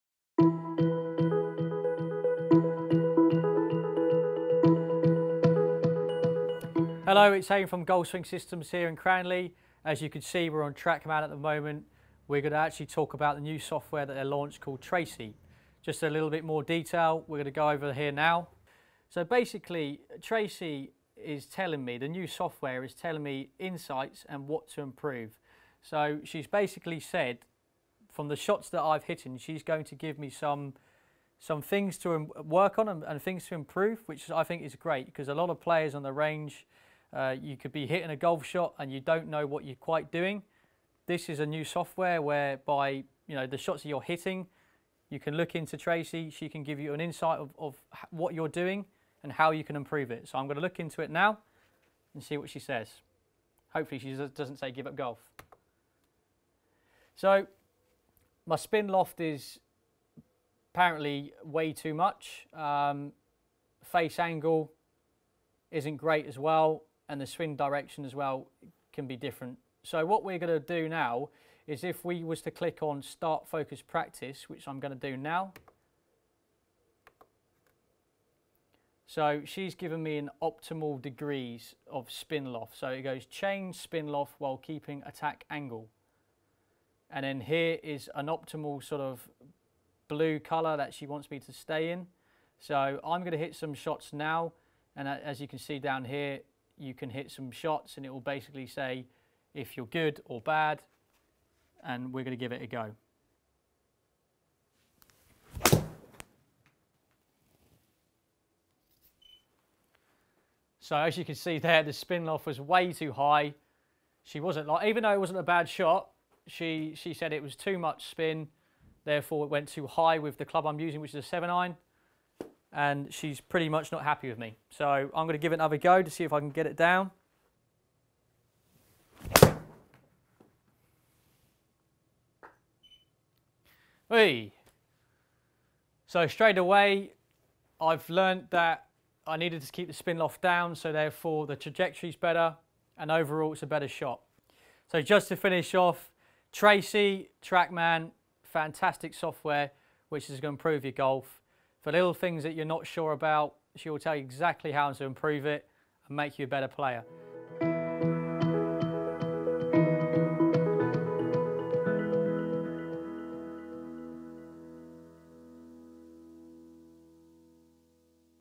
Trackman Tracy
Tour Professional Hadyn Reay shows Trackman's Artificial Intelligence software, Tracy.